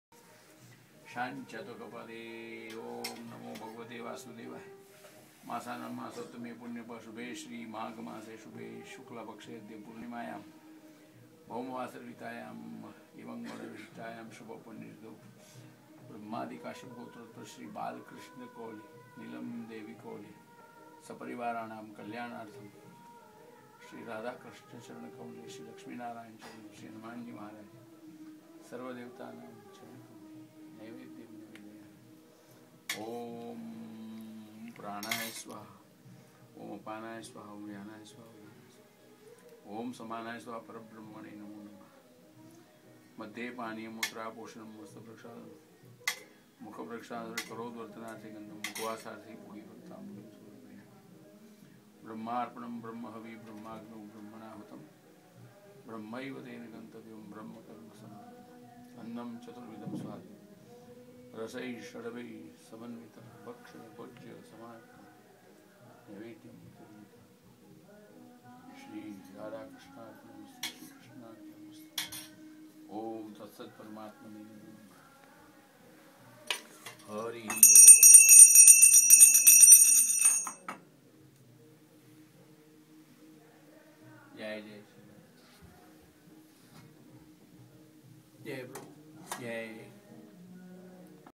Hindu Mandir Burnaby,B.C.Canada.
Live Bhog-offering to Prabu,before we start Langer in the temple by our respected Pandit Ji, in Hindu Mandir Burnaby.
Host family :--
Mr.Krishan Kohli & Mrs Neelam Kohli...